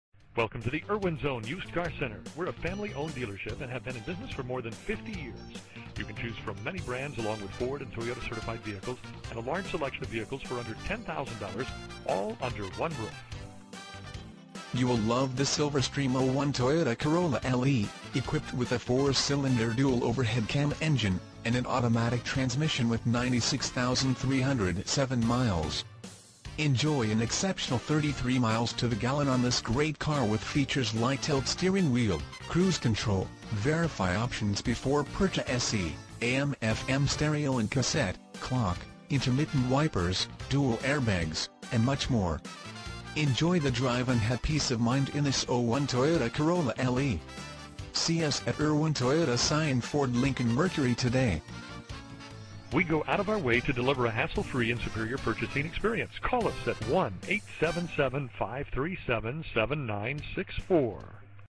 SOLD - 2001 Toyota Corolla LE 03246 Irwin Toyota Scion Ford
Irwin Toyota Scion Ford Lincoln Mercury

Bisson Avenue

2001 Toyota Corolla LE

 96,307 Miles

VIN - 2T1BR12E01C515455

Imagine driving this Silverstream 2001 Toyota Corolla LE, equipped with a 4 Cyl. DOHC engine  and an automatic transmission with  96,307 miles. Enjoy an exceptional 33 miles to the gallon on this great car with features like Tilt Steering Wheel, Cruise Control, Verify Options Before Purchase, AM/FM Stereo and Cassette, Clock, Intermittent Wipers, Dual Air Bags, and much more. Enjoy the drive and have peace ...